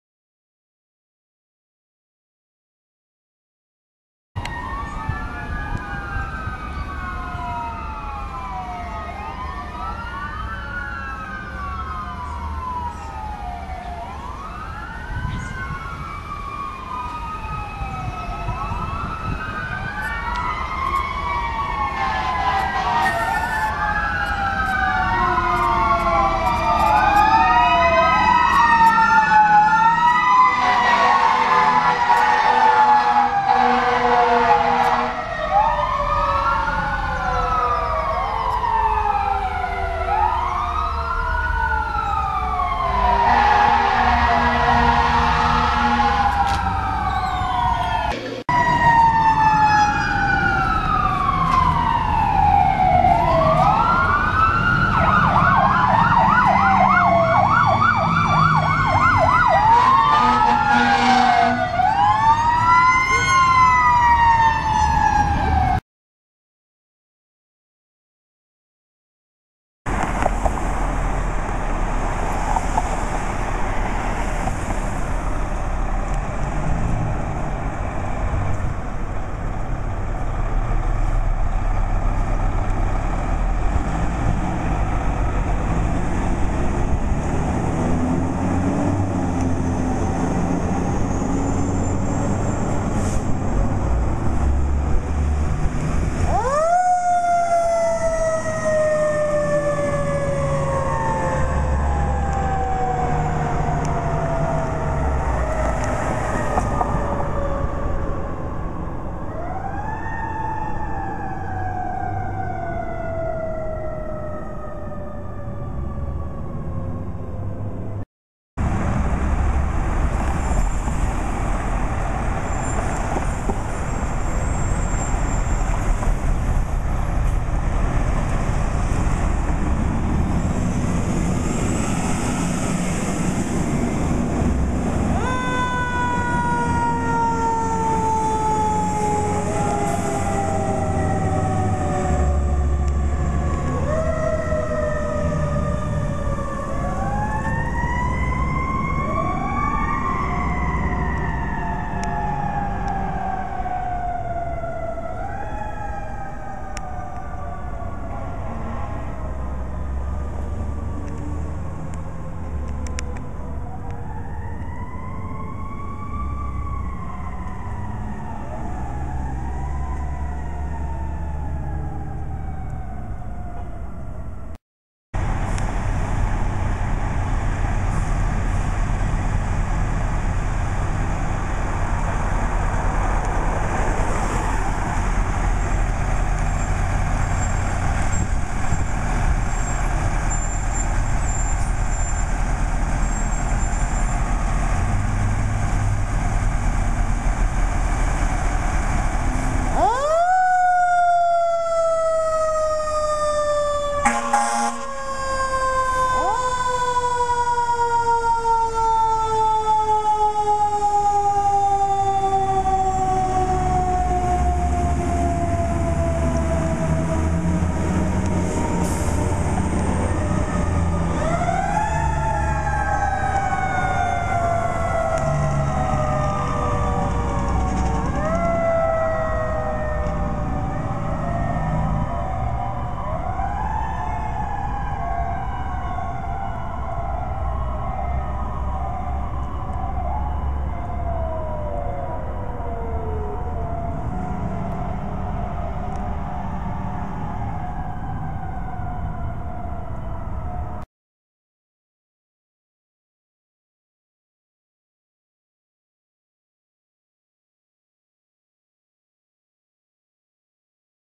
Video Compilation San Jose Fire Dept.
Response 1: Truck Company 18 Responds to 2 alarm fire

Response 2: Engine, and Truck Company 18 Responding to a call

Now it's time for Truck Company 35 Responses (All EMS Calls)

Response 3: Truck Company 35 Responds to an EMS call

Response 4: Truck Company 35 Responds to a medical

Response 5: Truck Company 35 Responding to an EMS call putting on a big sirenfest.